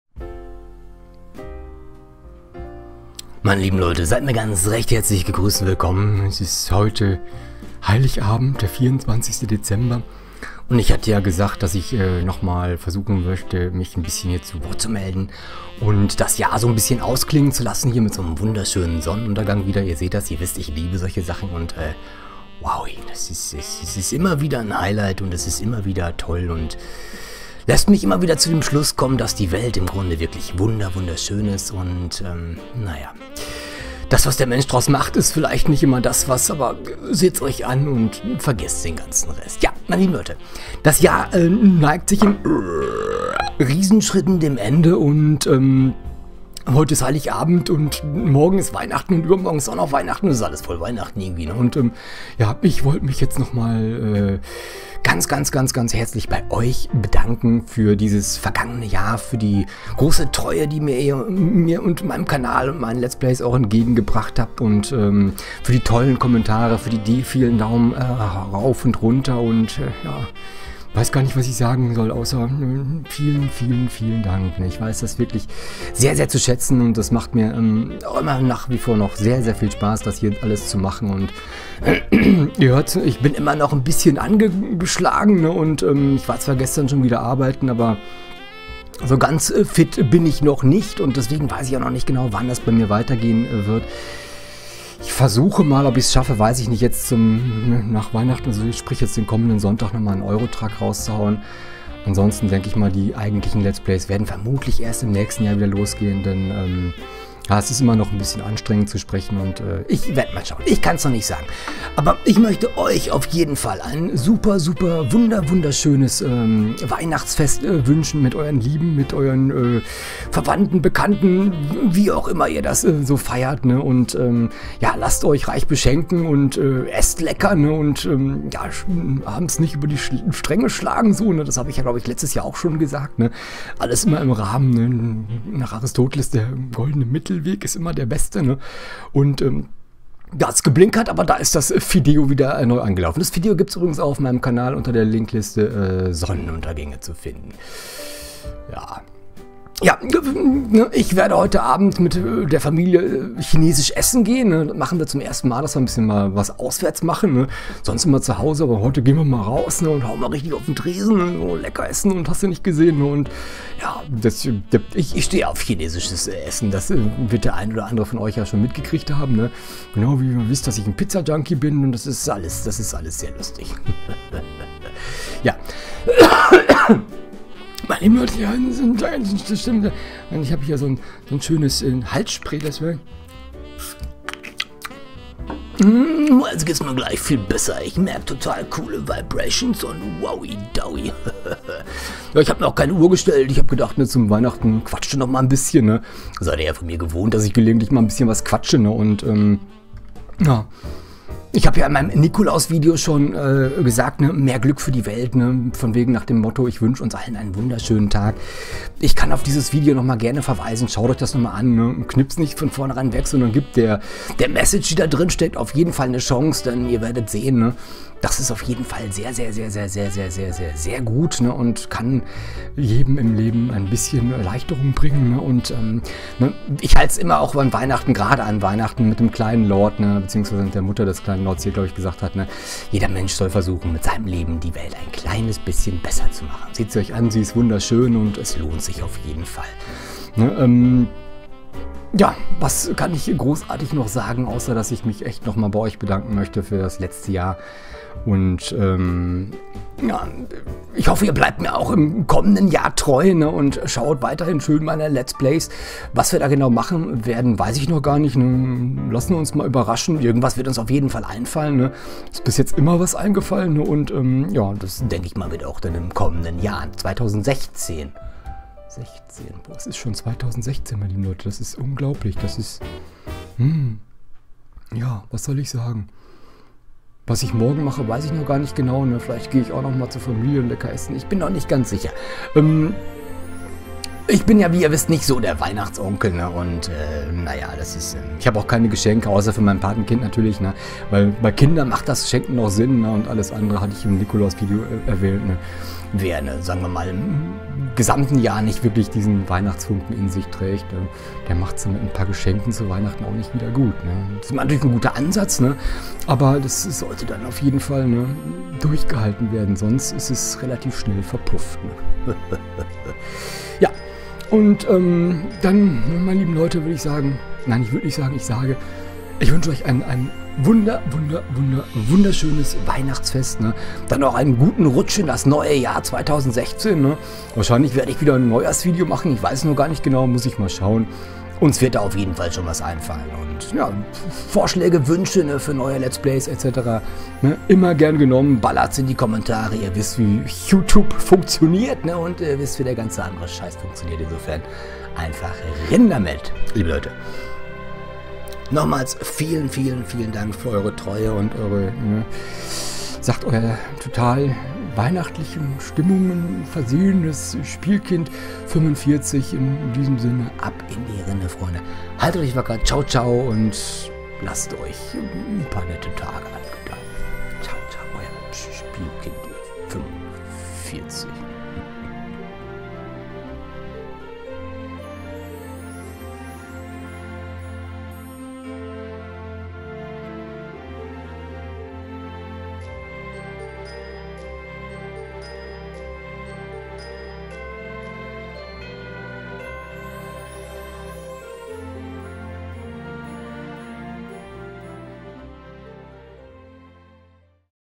Frohe Weihnachten und ein wunderschönes neues Jahr 2016 wünscht Euch Spielkind45
Liebe Leute,
wieder einmal ist Weihnachten! ;-)
Ich wünsche euch ein wunderschönes Weihnachtsfest und einen guten Rutsch ins neue Jahr 2016!
Euer
Spielkind45

Über einen Daumen RAUF würde ich mich sehr freuen!
Und ABO gibt's natürlich gratis! Also ran an den Button! ;-)